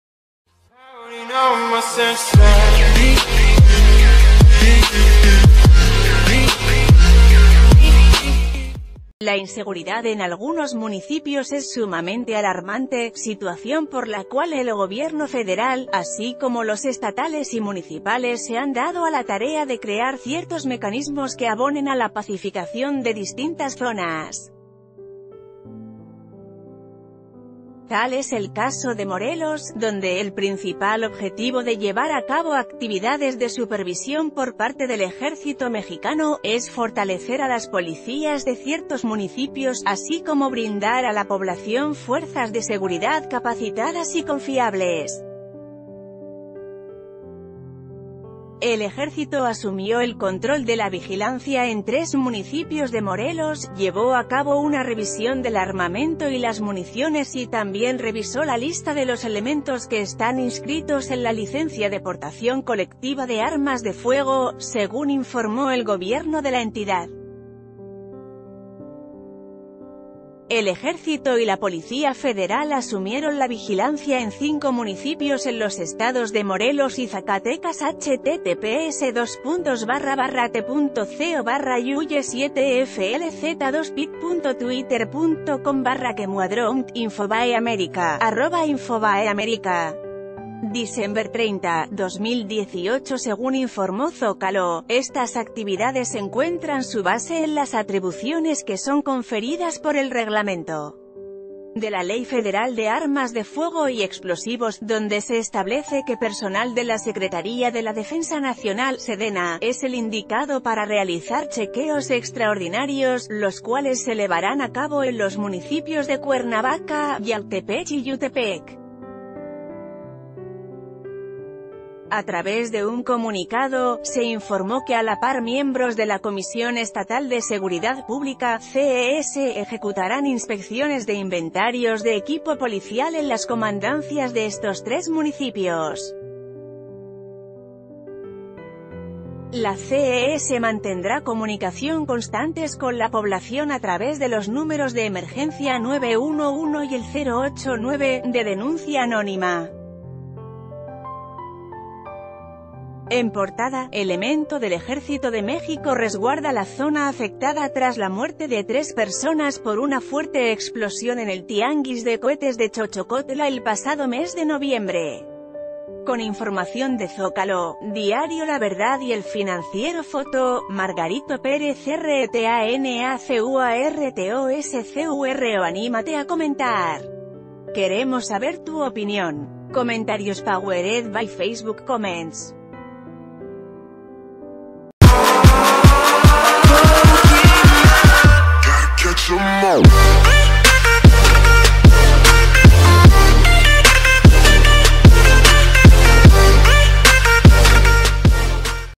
Ejército toma el control de la vigilancia en tres municipios de Morelos
La inseguridad en algunos municipios es sumamente alarmante, situación por la cual el Gobierno Federal, así como los estatales y municipales se han dado a la tarea de crear ciertos mecanismos que abonen a la pacificación de distintas zonas. Tal es el caso de Morelos, donde el principal objetivo de l...